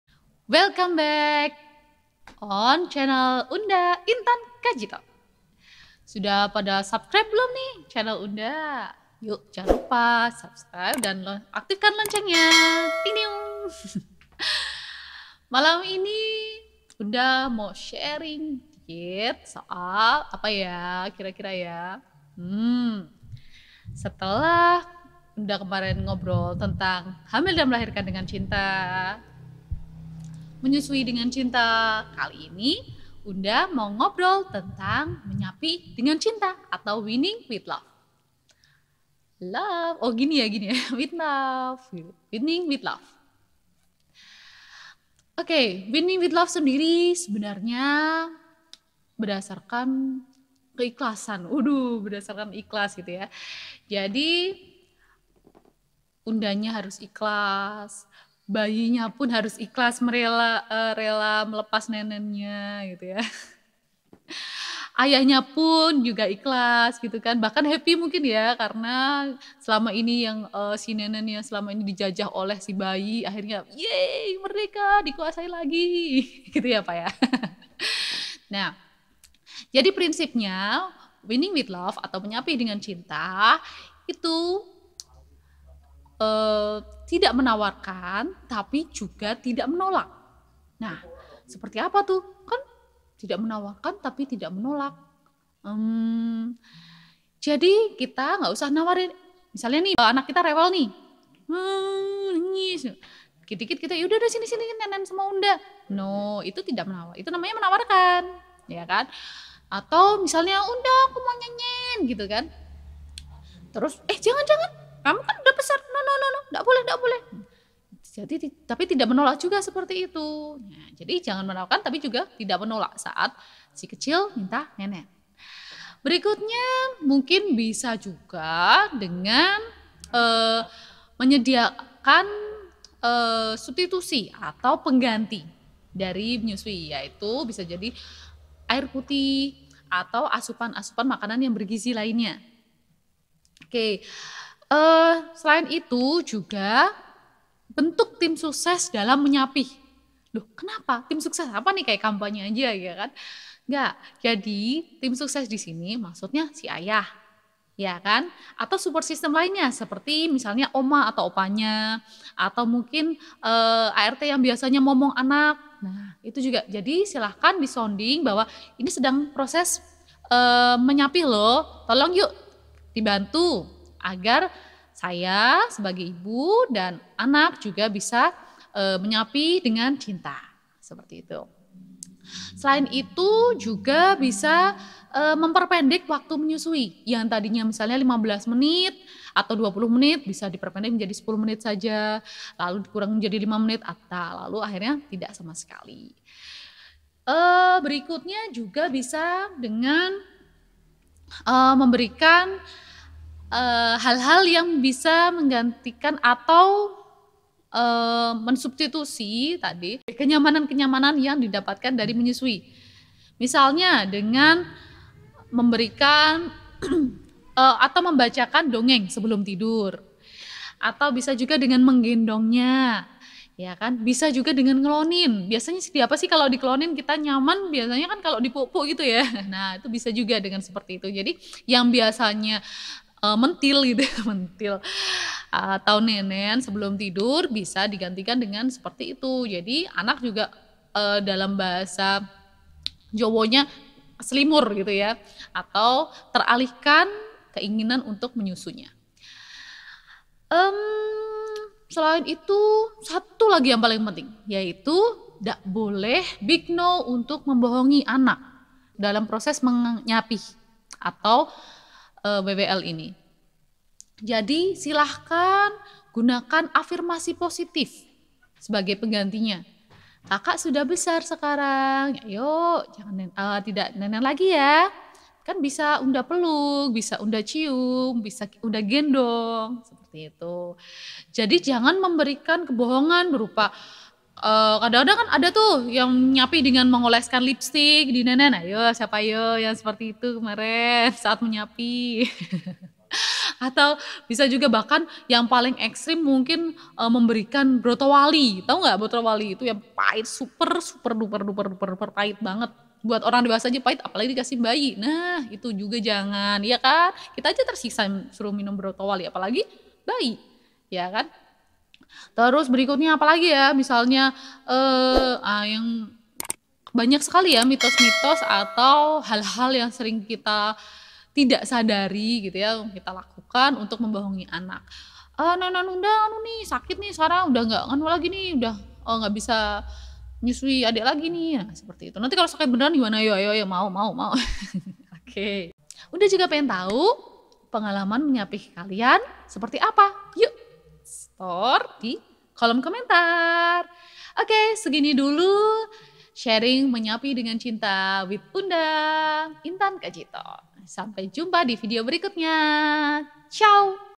Menyapih dengan Cinta Tanpa Drama Kumbara
Mana suaranya yang masih bergulat dengan drama menyapih?
Cuuungg..

Samaaaa, hahaa..
Tapi bedanya tanpa drama kumbara yang melanglang buana.

Oles lipen?
Kasih brotowali?
Atau dijopa-japu alaehum gambreng jadi berhenti nenen?

Nooo, menyusui dengan cinta, nyapih pun kudu dengan cinta dong 😍

Seperti apa sih menyapih dengan cinta?
Yuuk kita ngobrol tentang WEANING WITH LOVE di sesi ini.

Boleh banget share pengalaman menyapih buah hati di kolom komentar ya, Mommies 😘

Mommies bisa follow juga instagram Unda @intan_kajito, biar makin intens 🤗